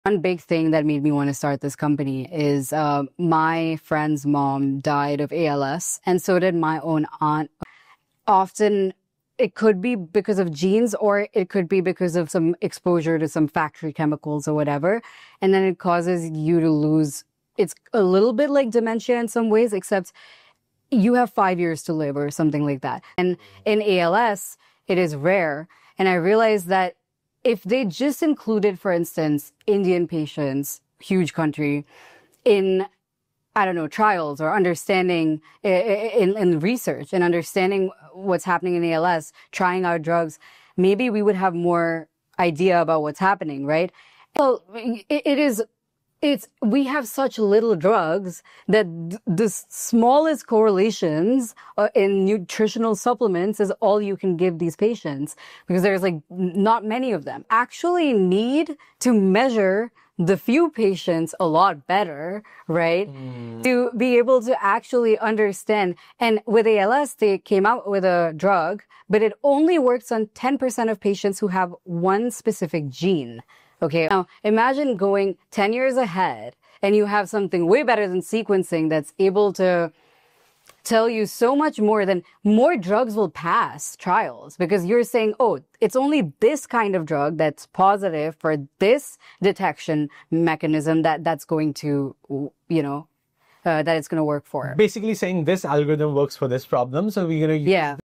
ALS: New Drug Trials & Future of Treatment
ALS is a devastating disease, often linked to genes or chemical exposure, robbing patients of their abilities. Better research, drug trials, and understanding, especially including diverse populations, are crucial for progress and new treatments.